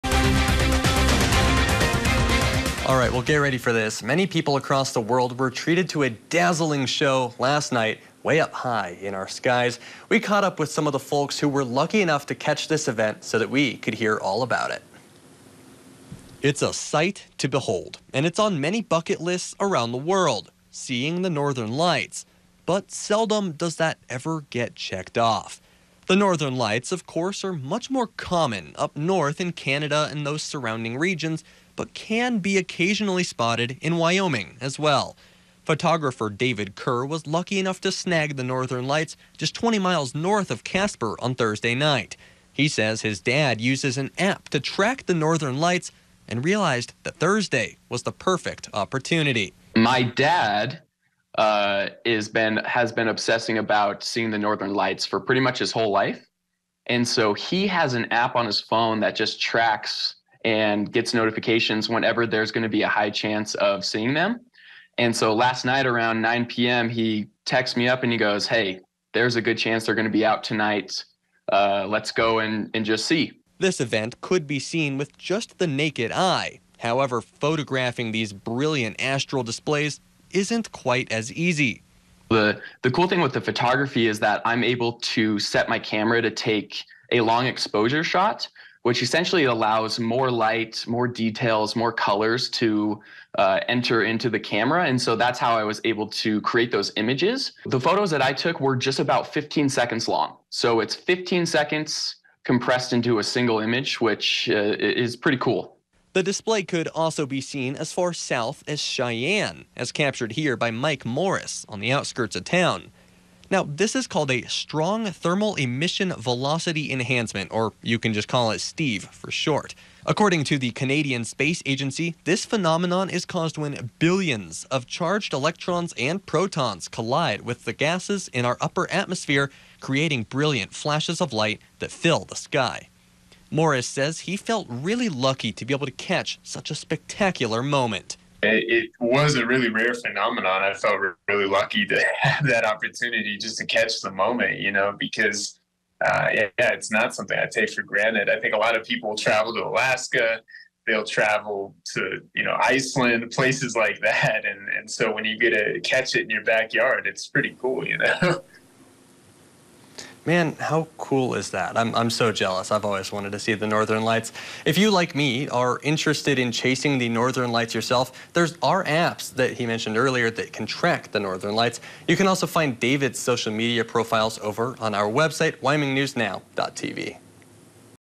Wyomingites Dazzled By Northern Lights
ASK ANY OF YOUR FRIENDS WHO TRAVEL WHAT THEY WANT TO SEE MORE THAN ANYTHING ELSE.

AURORA BOREALIS WILL LIKELY BE MENTIONED WITHIN A FEW SECONDS.

SOMETIMES, THOUGH... YOU DON'T HAVE TO GO AS FAR AWAY AS YOU THINK TO SEE THE PHENOMENON.

AIDEN GRECO REPORTS.